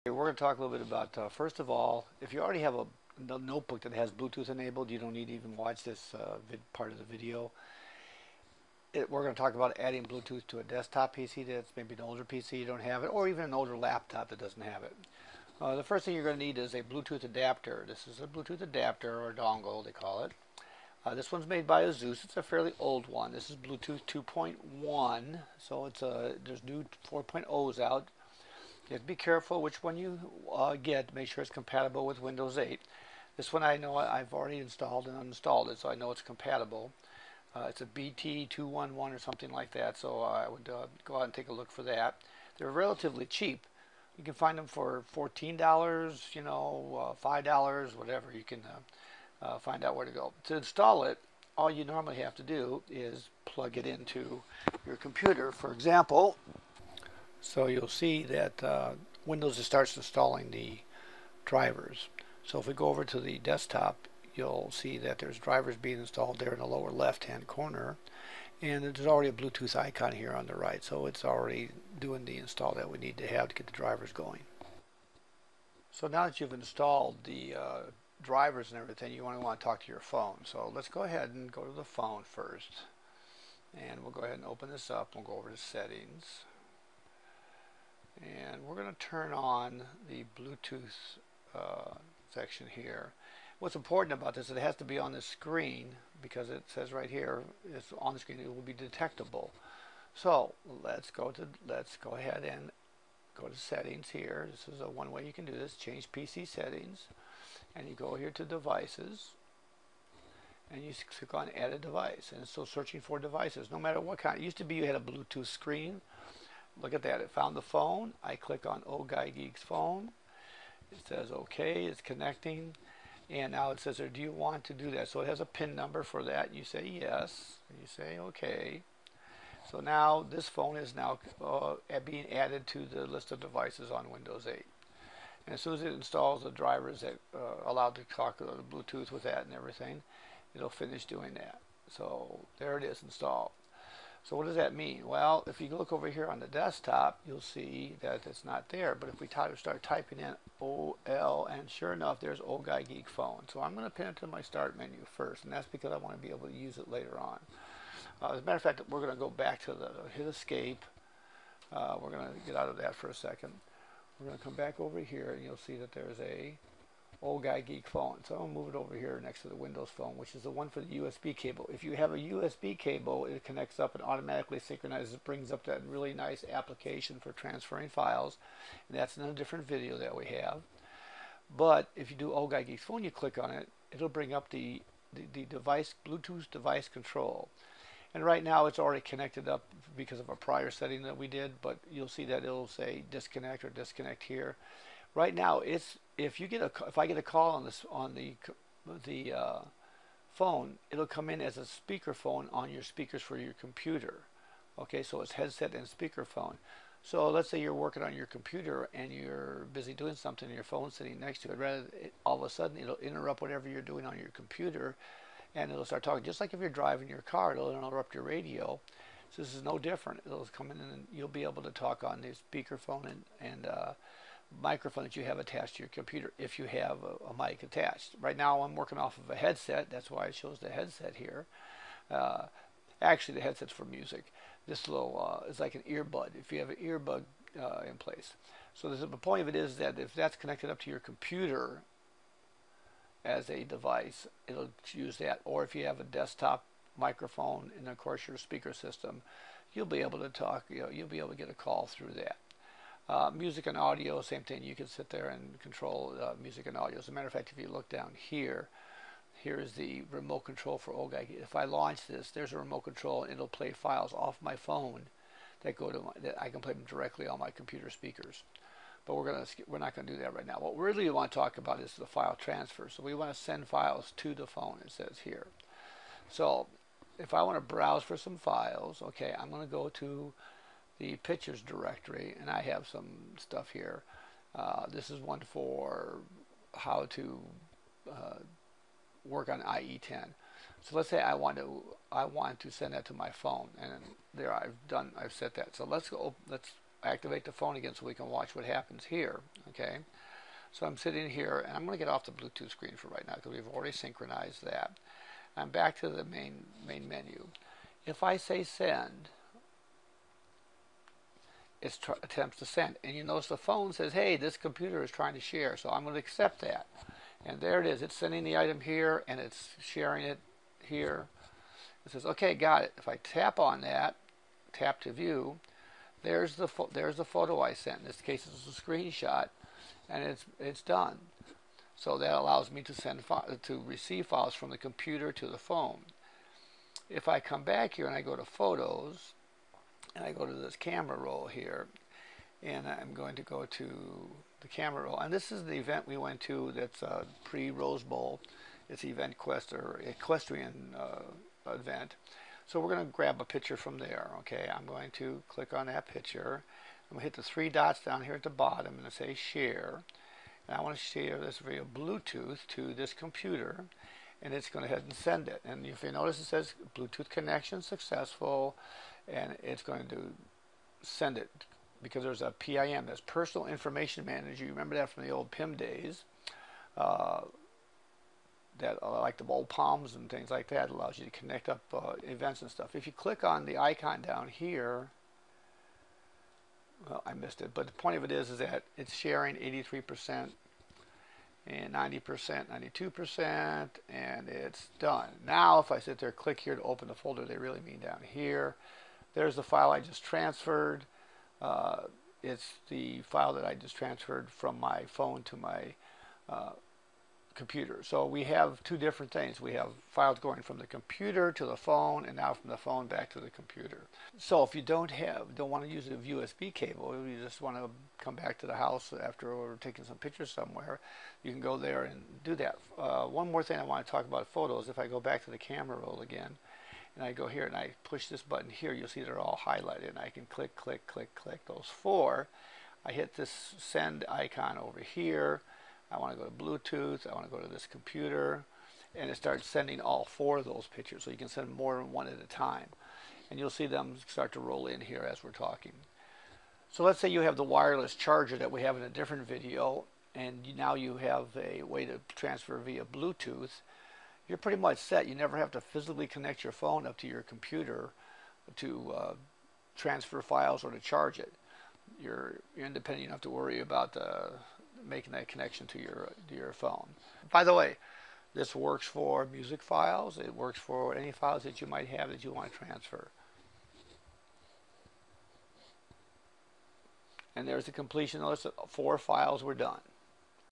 Windows Phone 8 Transfer Files with Bluetooth
You never have to plug in your USB cable to transfer your files to and from your phone.  Although this video demonstrates a Windows 8 systems and a Windows 8 phone, you can do the same thing with Windows 7 and other Bluetooth devices.

Once you establish a Bluetooth pairing, it just takes a few clicks to control which pictures, videos or music you transfer.

The Windows Phone 8 utility that we demonstrate in another video only works with a USB connection, at least for now.  There also are some other settings that allow you to autosync files as well.

Our next file transfer video will discuss SkyDrive and how you can use it with your phone and your PC.  After all it's the Cloud.